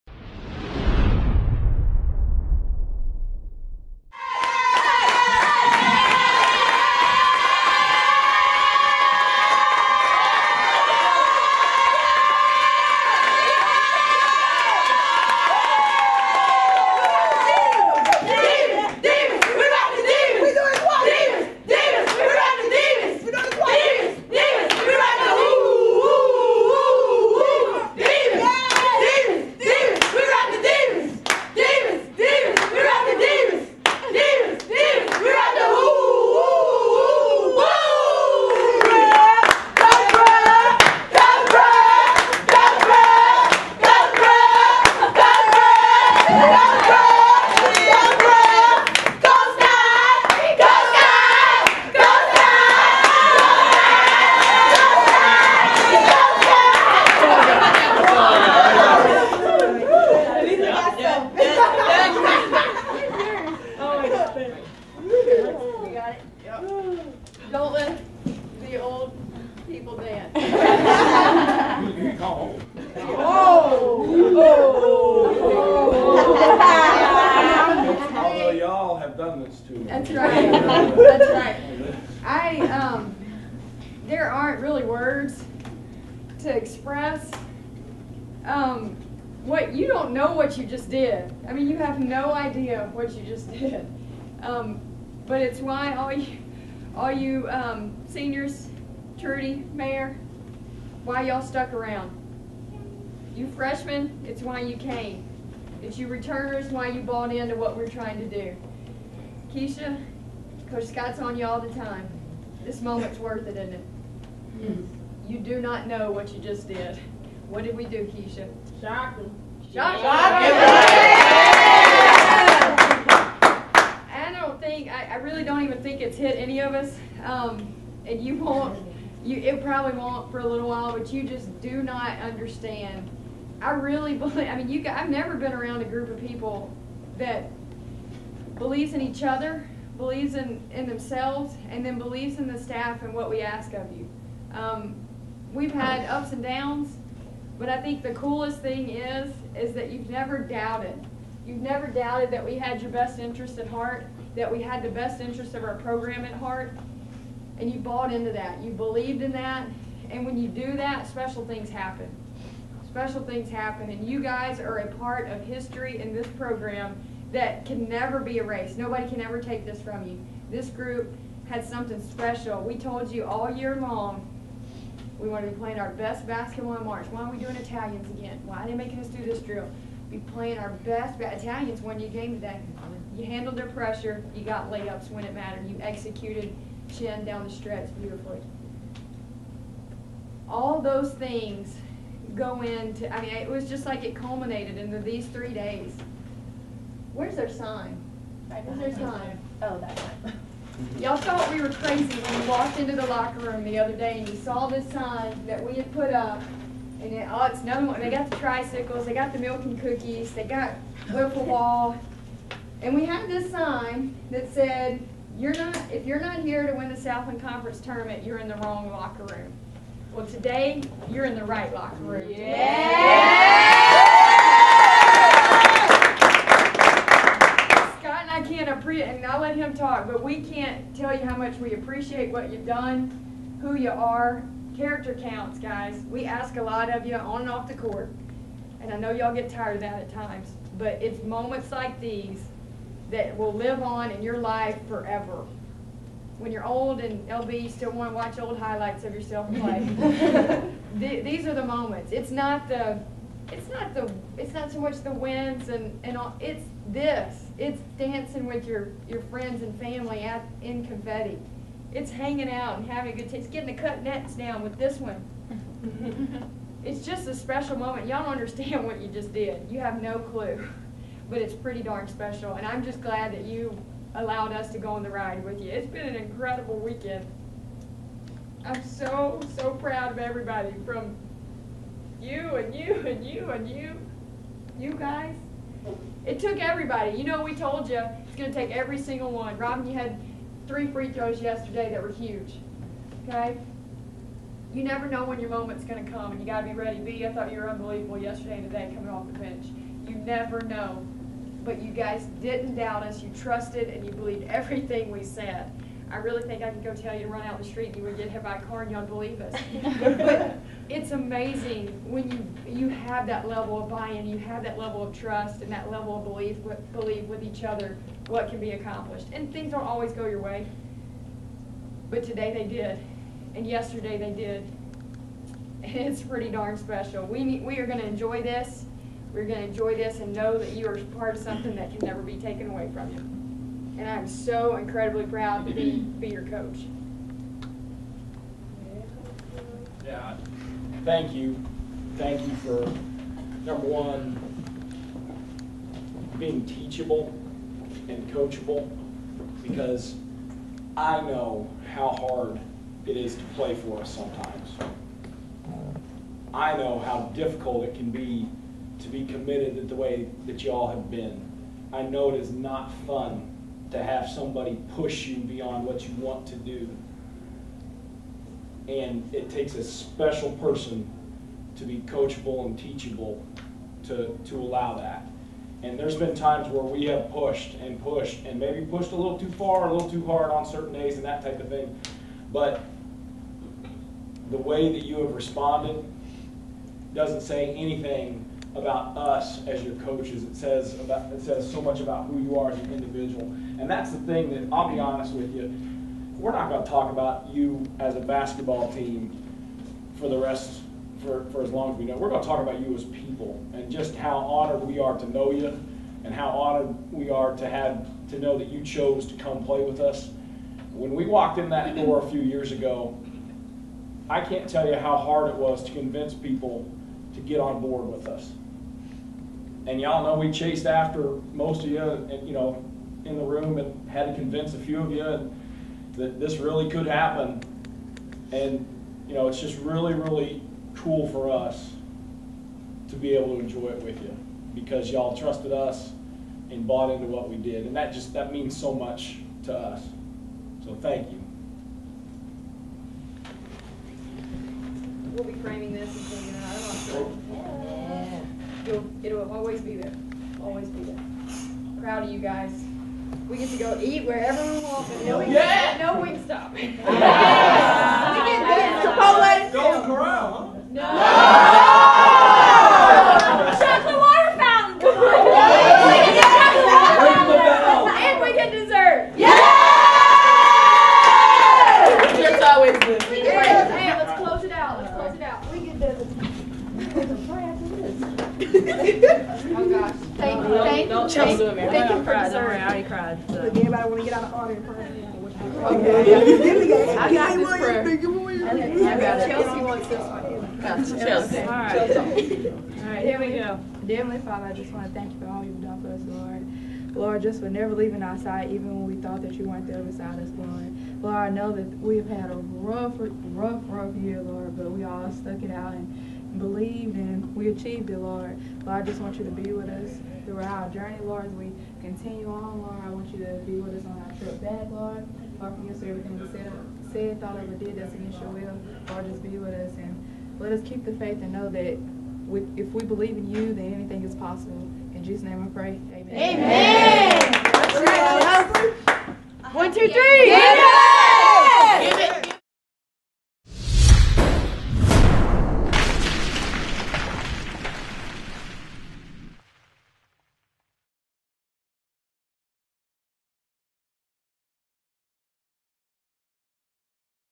All-Access: Lady Demons Championship Locker Room
Go inside the Northwestern State women's basketball locker room following the Lady Demons' Southland Conference championship game victory. Watch some dancing by coaches Brooke and Scott Stoehr, an emotional speech and a team prayer.